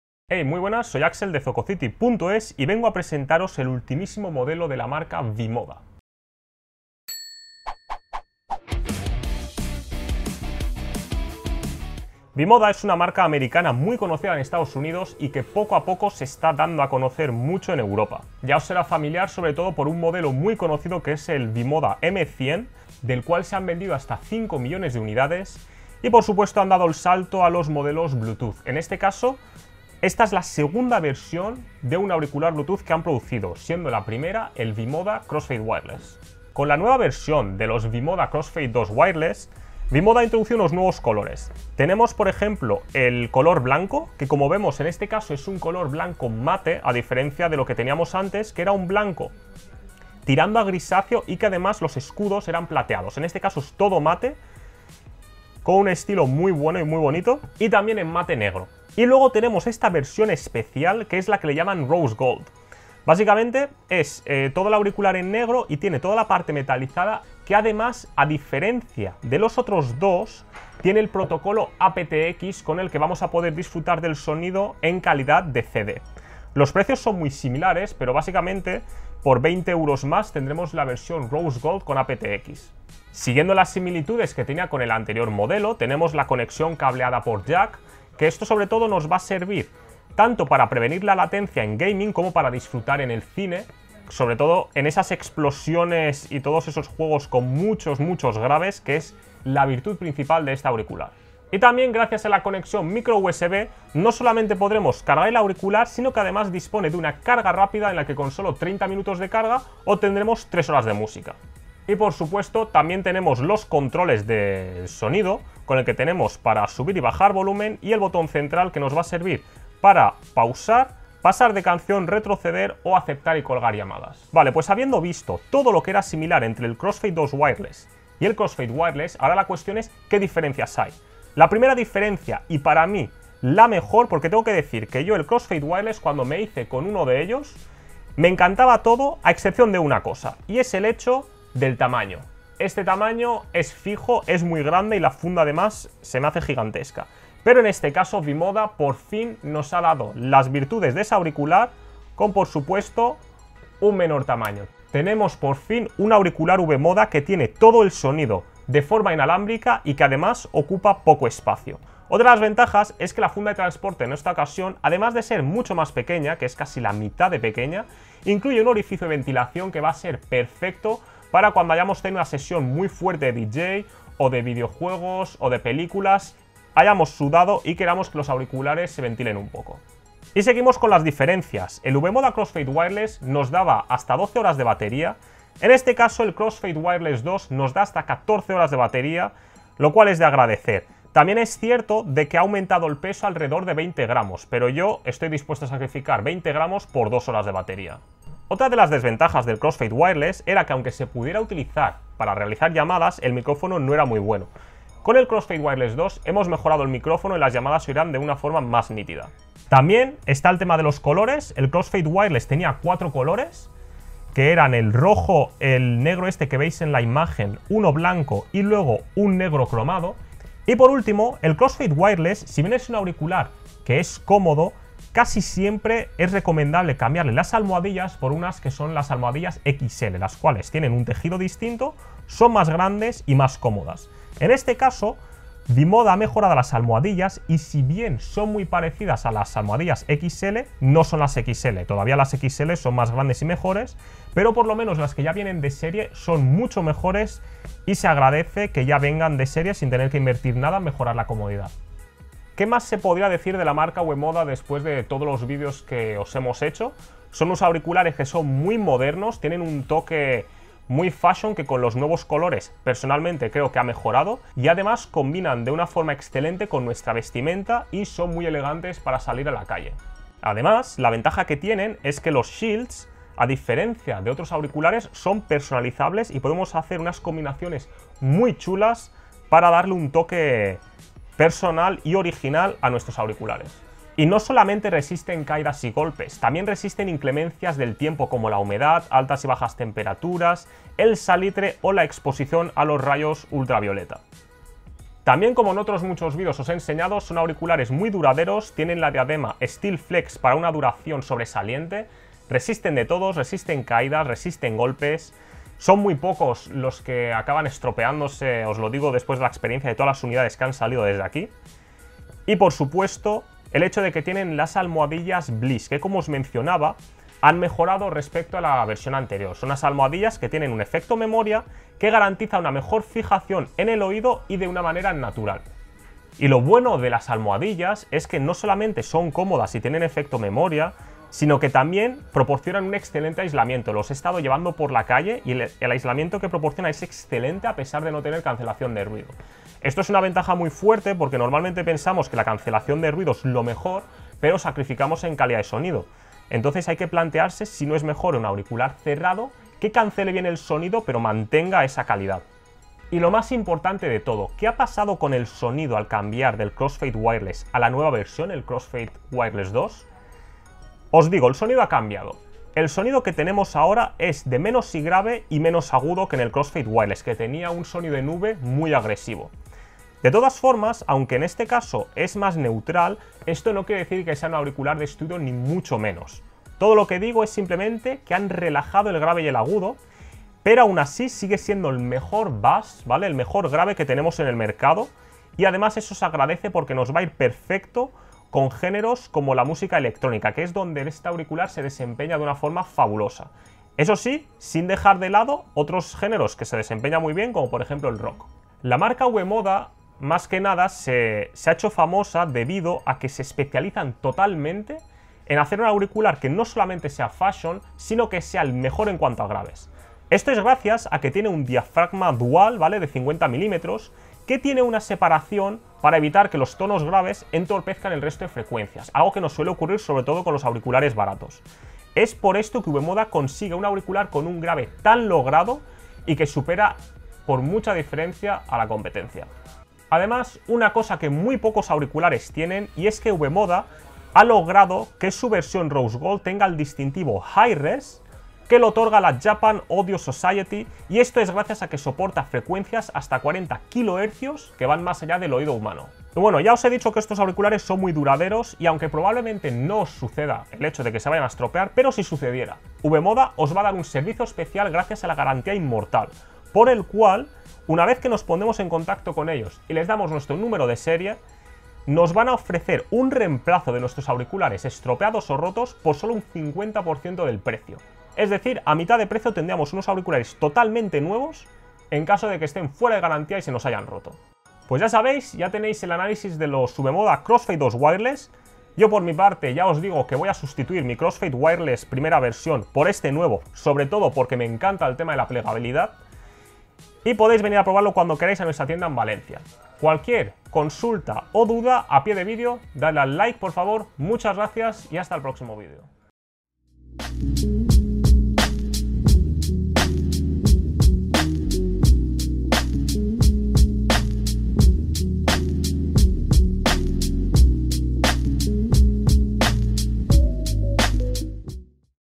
Review V-Moda Crossfade II Wireless, los Crossfade renovados!
V-Moda ha decidido renovar uno de sus modelos más exitosos V-Moda Crossfade Wireless con el lanzamiento de su nueva versión que integra nuevos colores y mejoras sustanciales en algunos de sus acabados como la introducción del protocolo aptX.

► Comprar V-Moda Crossfade II Wireless al mejor precio. Distribuidor oficial